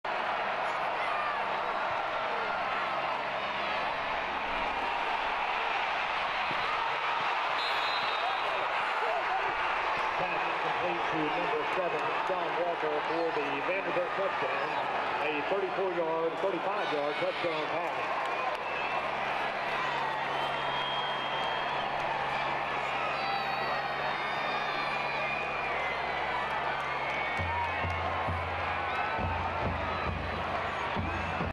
Sean Walker TD catch
Vanderbilt receiver Sean Walker 35yard TD catch versus UGA